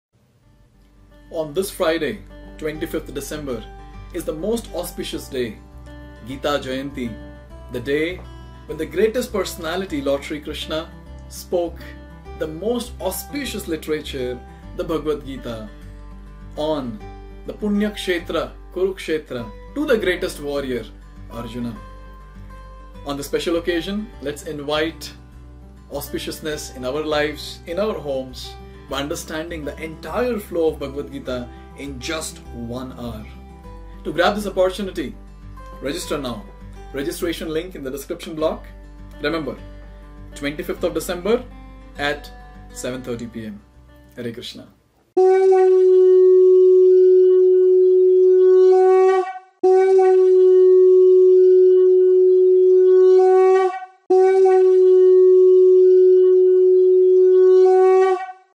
Full Overview Of Gita, in just 1 hour | Kartikeya Das
This session is also conducted in 8 other languages
मराठी | ଓଡିଆ | ગુજરાતી | ಕನ್ನಡ | বাংলা | தமிழ் | हिंदी |  తెలుగు.
For registration visit register.gogita.in